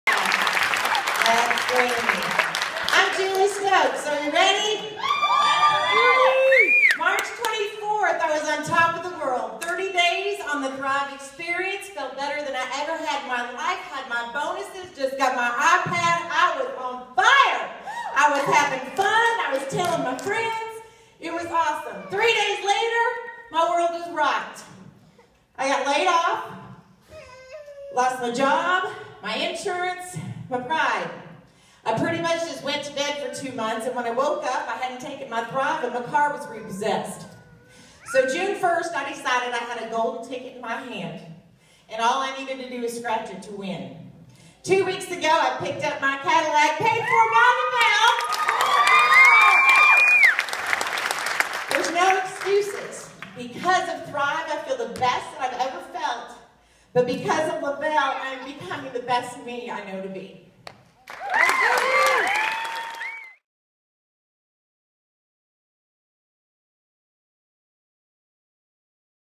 My Thrive Experience in 60 Seconds Julie Stokes
Quick Thrive experience video recorded durning a Le-Vel Local in Krum, Texas USA Novermber 2014.   Find me beachdreammarketing.net